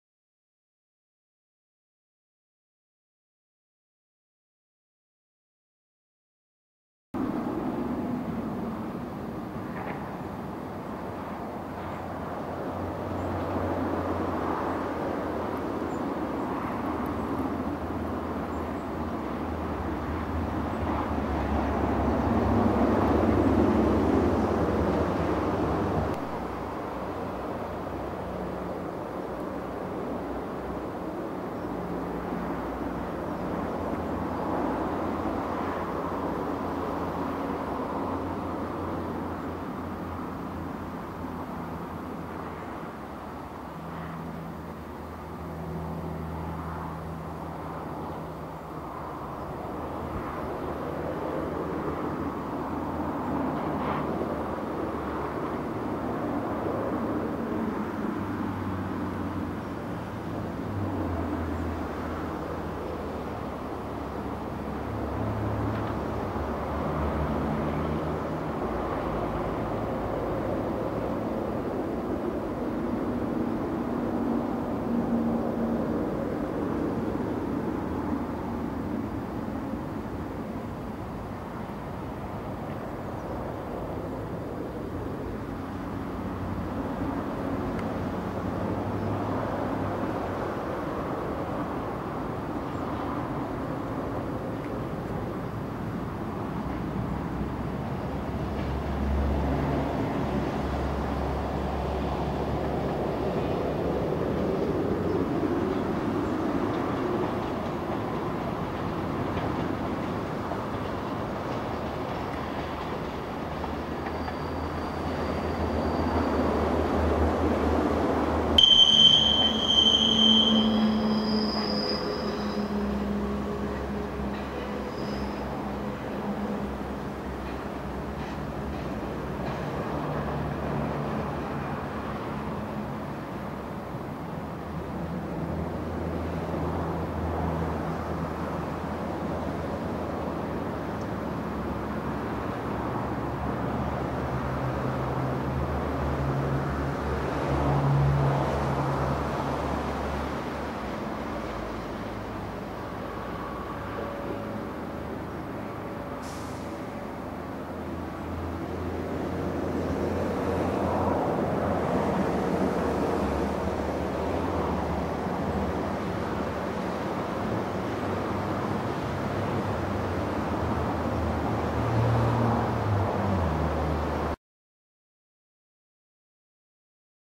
Railways Newcastle KEB 28th September 1997
First time out with new digital video camera at the south end of the KEB Newcastle.